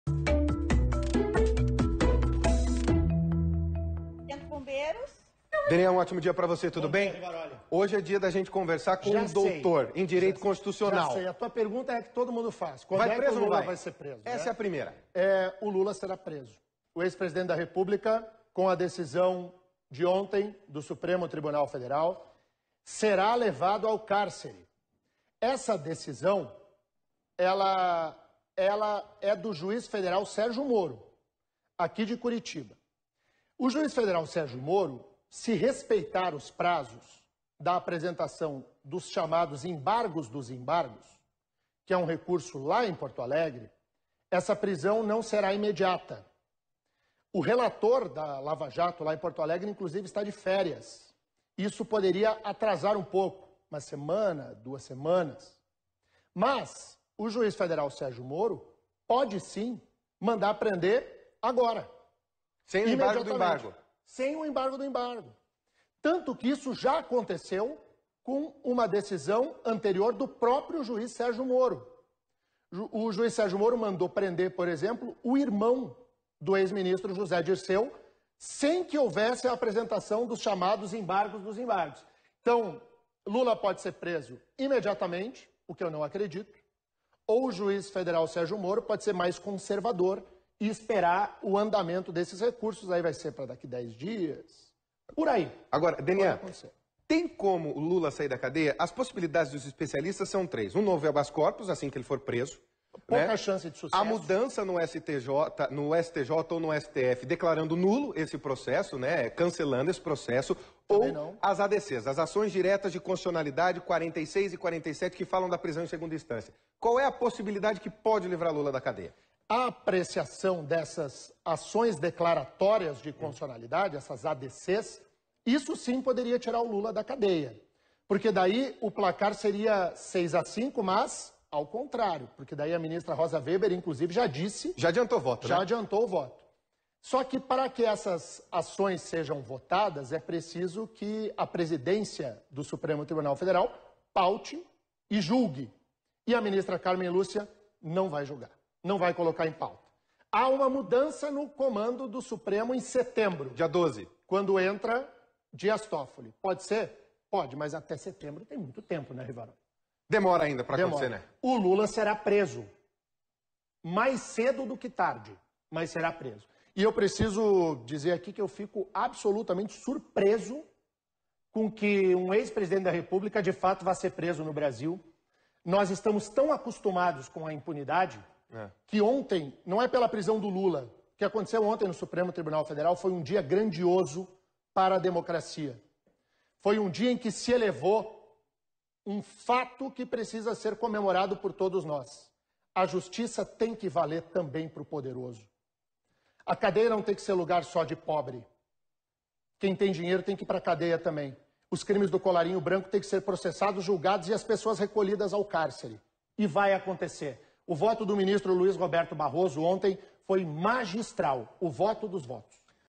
Opinião RIC: Denian Couto fala sobre decisão do STF e possível prisão do ex-presidente Lula
Para Denian, o ex-presidente Lula será preso com a decisão dos ministros do STF de rejeitar o pedido do habeas corpus.